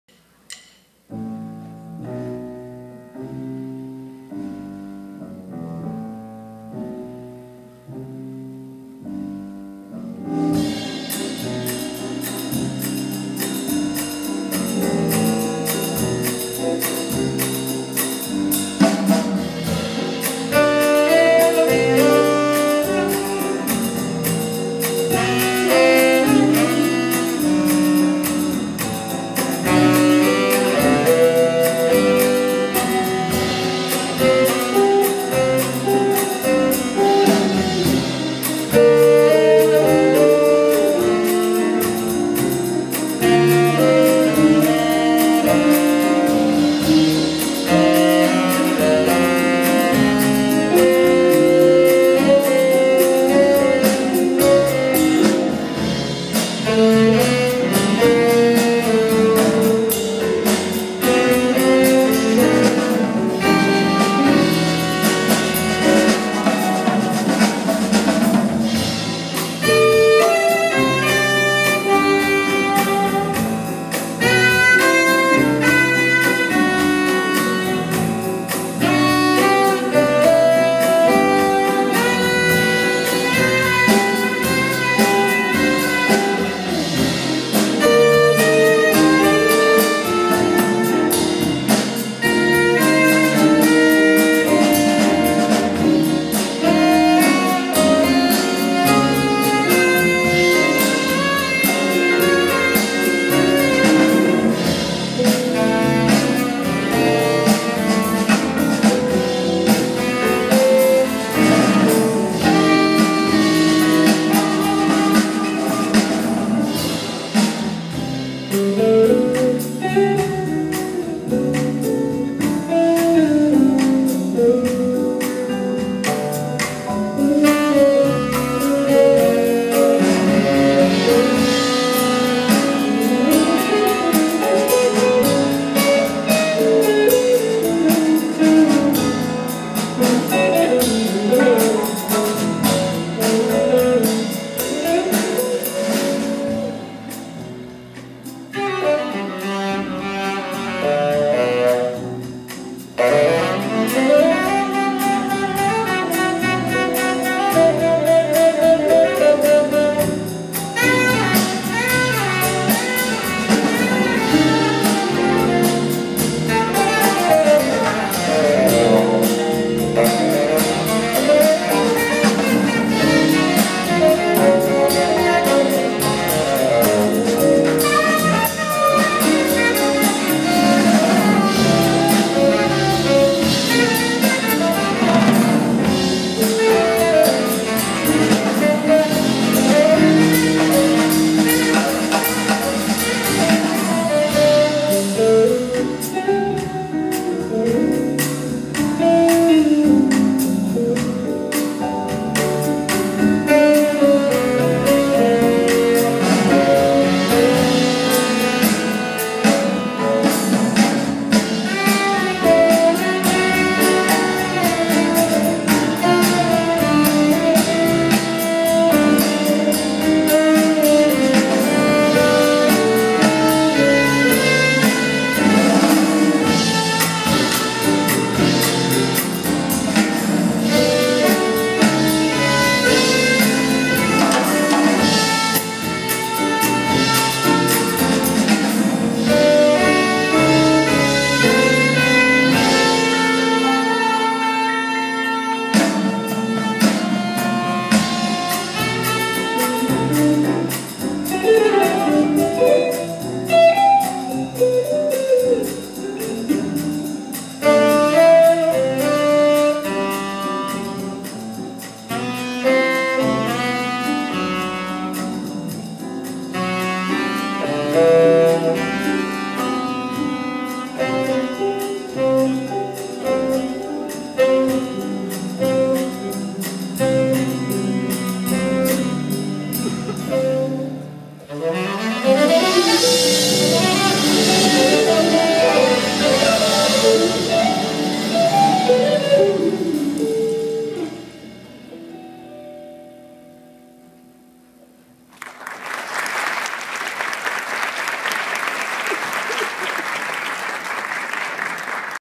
BERNARDO FABRIS QUINTETO -
Teatro Sesiminas, Belo Horizonte, MG.
15 de maio de 2015.
Bernardo Fabris: sax tenor
Marcos Abjaud: piano
Yan Vasconcellos: contrabaixo
Hudson Vaz: bateria
???: guitarra